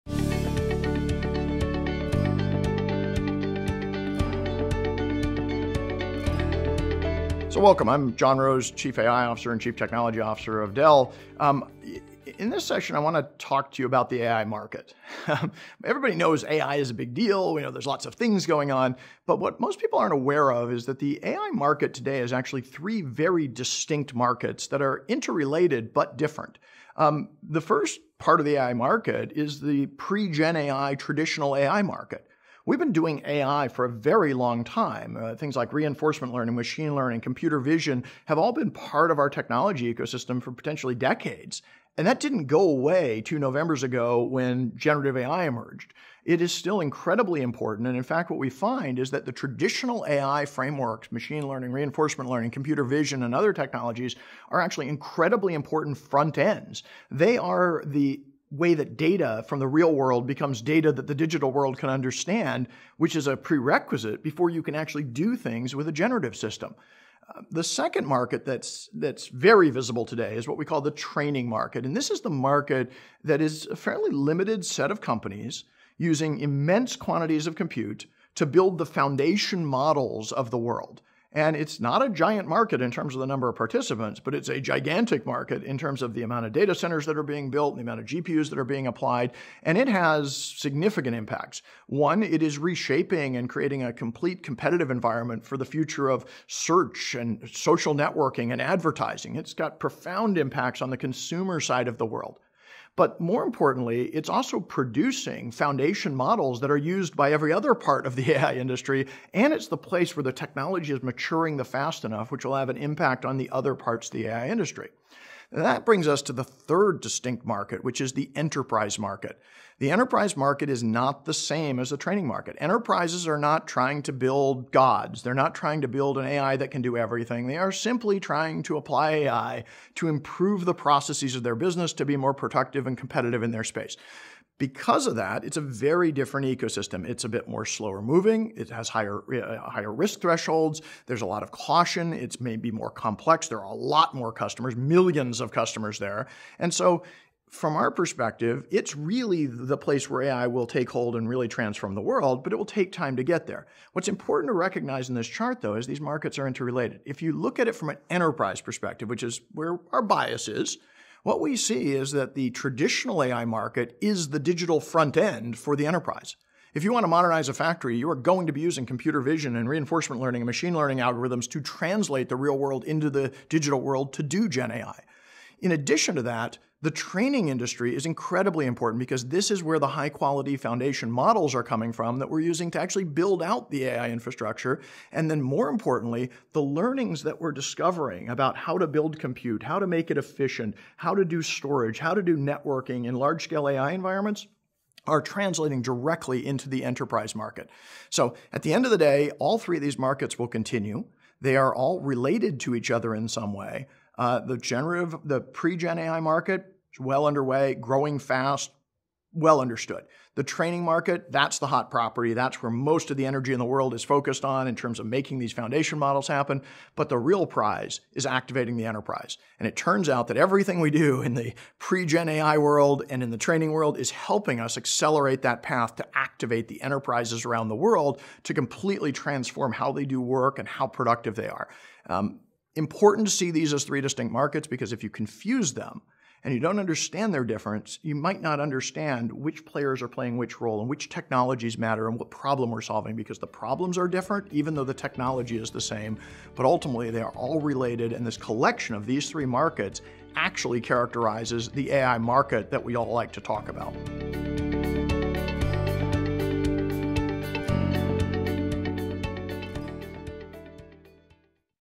The Three AI Markets Shaping Our Future
Three Artificial Intelligence (AI) markets, one transformative future. Join John Roese, Dell Technologies Global CTO and Chief AI Officer, as he reveals how traditional, training and enterprise AI markets align to reshape industries—and why unlocking the power of enterprise AI is the real prize driving innovation and transformation.